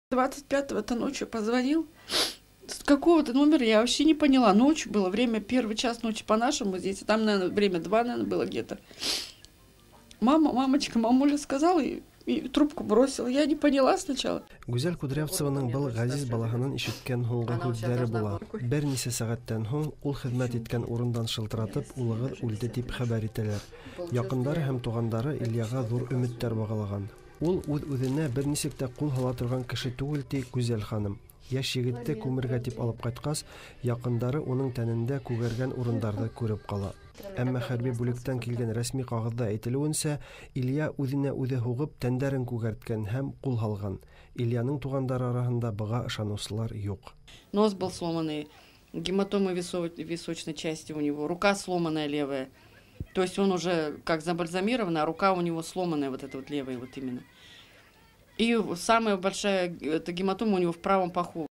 Башкортстан егете хәрби хезмәттән табутта кайтты
Башкортстанның Салават районы Мөрсәлим авылы егете Илья Кудрявцев Алтай краенда хезмәт иткән. Соңгы шалтыратуында елый-елый: “Әни, әни”, дип кенә әйтә алган. Рәсмиләр сүзенә караганда, егет аяк киеменең бавына асылынып үлгән. Мәрхүмнең әти-әнисе моңа ышанмый.